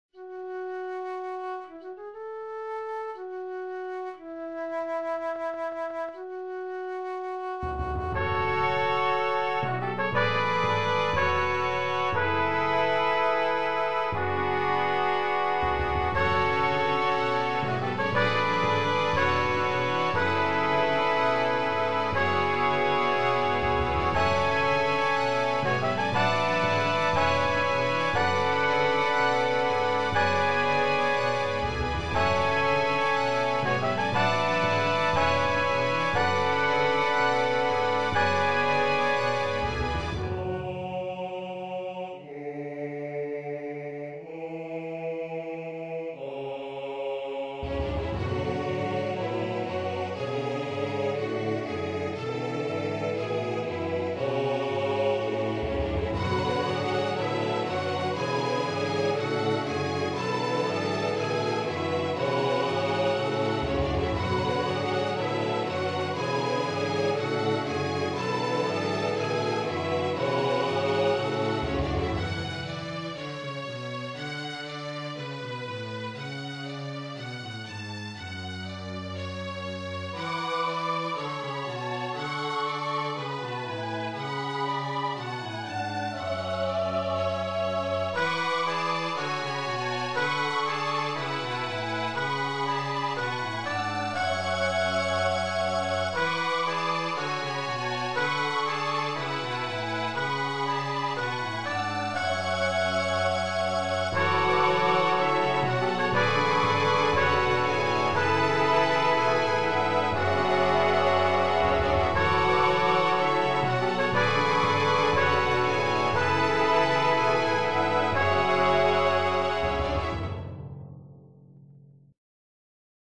Untitled Orchestra piece
An untitled piece where I basically just picked random instruments to write for. Call it an excersice if you will. Or at least an "Incomplete Orchestra Piece".